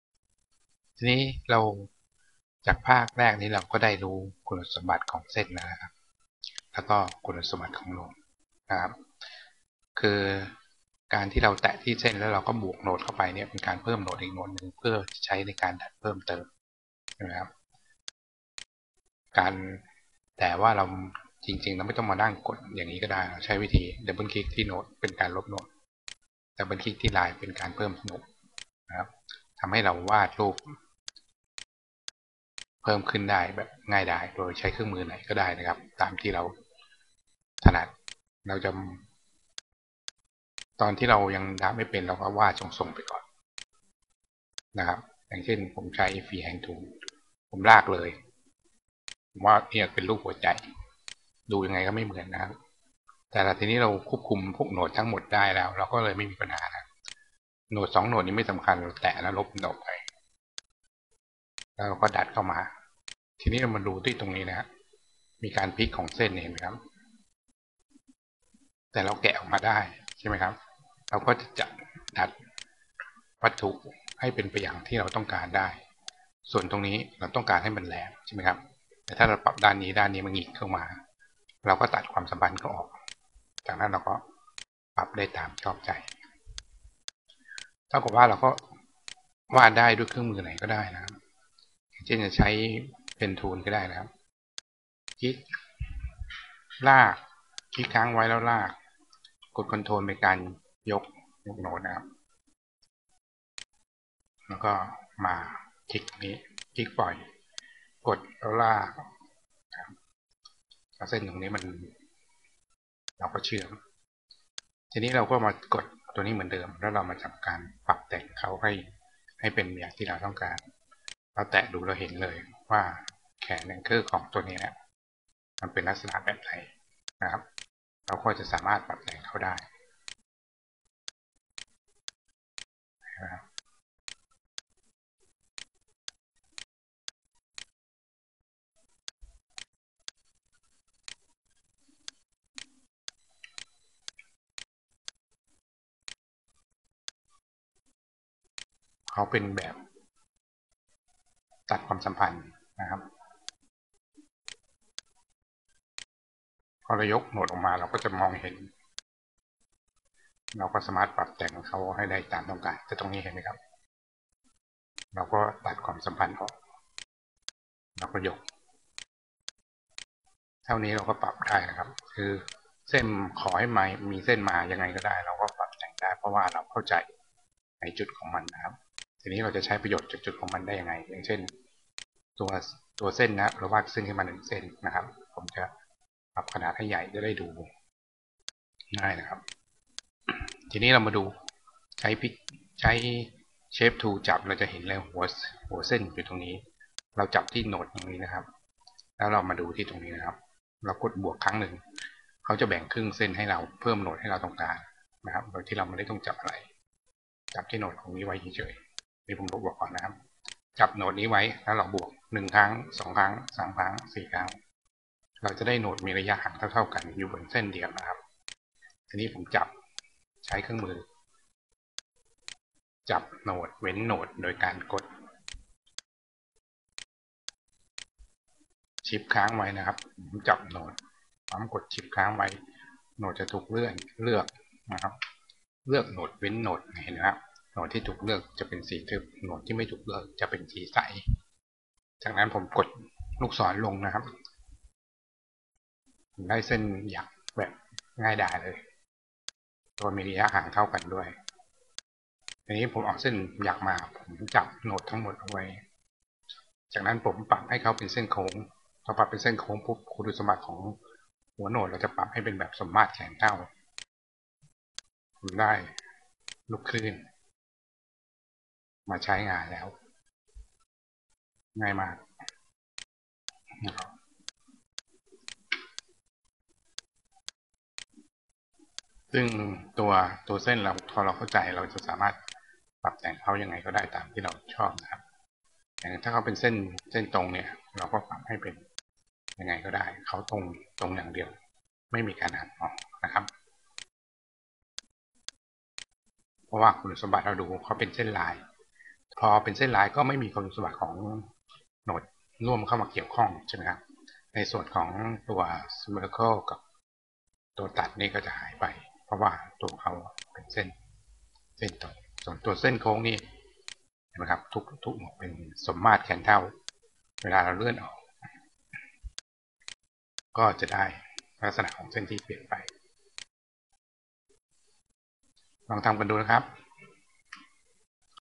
3.Line & Node in CorelDRAW 2/6
อธิบายเรื่อง เครื่องมือต่างๆ ที่ใช้ในการวาดเส้น แล้วปรับแต่ง
จากตัวอย่างใช้คุณสมบัติโค้งของโหนดวาดเป็นลูกคลื่น